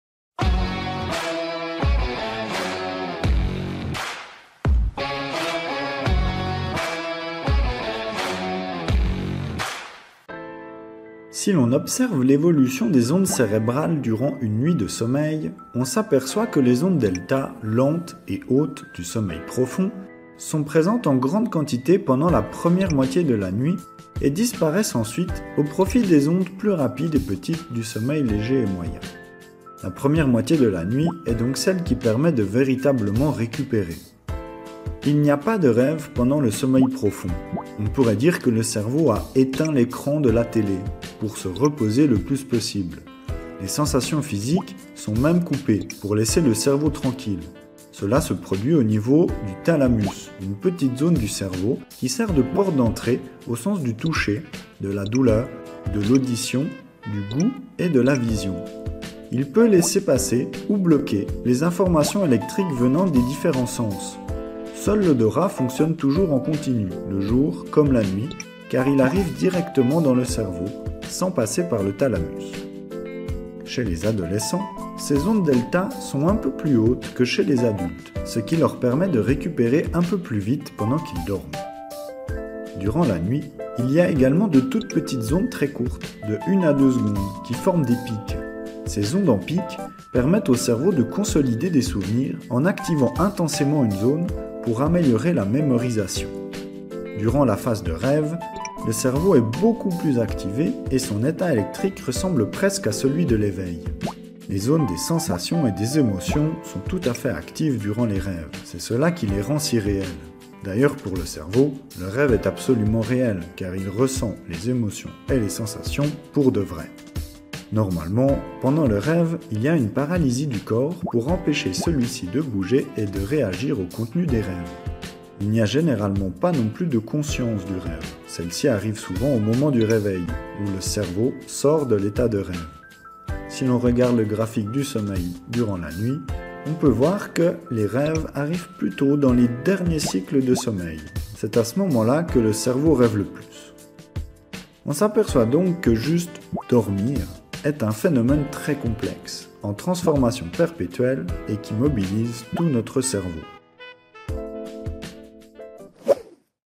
Le sommeil 2 : les rêves, ondes delta, pics d'onde...
Le contenu des vidéos est uniquement destiné à des fins d'information ou d'éducation et ne remplace en rien les consultations avec des professionnels de la santé. Sources :
Neural Circuitry of Wakefulness and Sleep (Thomas E Scammell , Elda Arrigoni, Jonathan, Lipton)
Sleep in adolescence: Physiology, cognition and mental health (Leila Tarokh, Jared M Saletin, Mary A Carskadon)